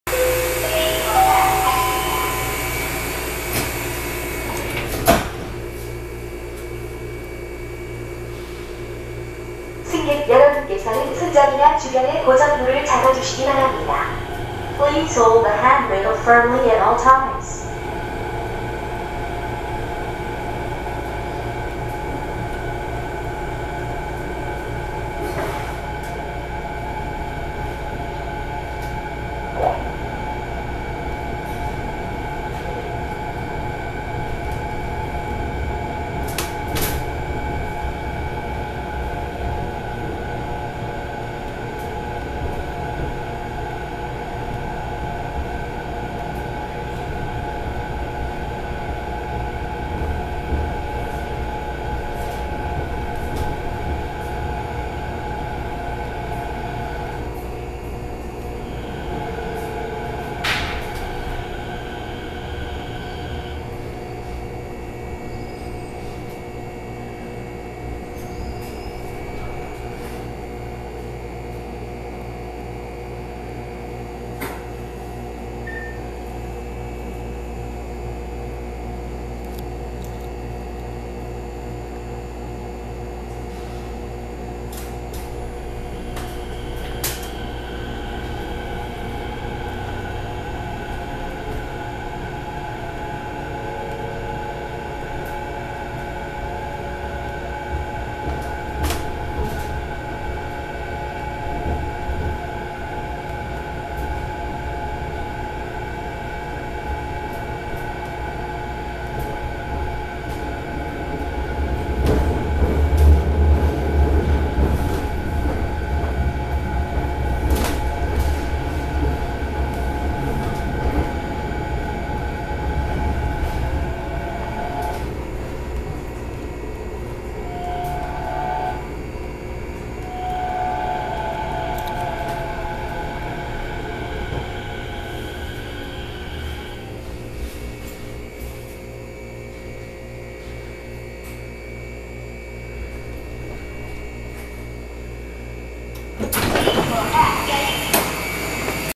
부산김해경전철 회송열차 가야대역 → 가야대역 회차선 → 가야대역
Busan-Gimhae Light Rail Transit - Forwarding Train, Kaya Univ. Station → Kaya Univ. Station Turn Line → Kaya Univ. Station

촬영에 앞서 환경미화원분께 탑승 허가를 구하고 촬영된 영상입니다. 오해 없으시기 바라며 행선지는 애매해서 가야대역 LED에 나오는대로 회송이라고 표기를 했습니다.

* 2018년 6월 30일 촬영

* 채널의 모든 영상은 재업로드를 금지합니다. 영상을 사용하실때에는 출처를 꼭 남겨주시기 바랍니다.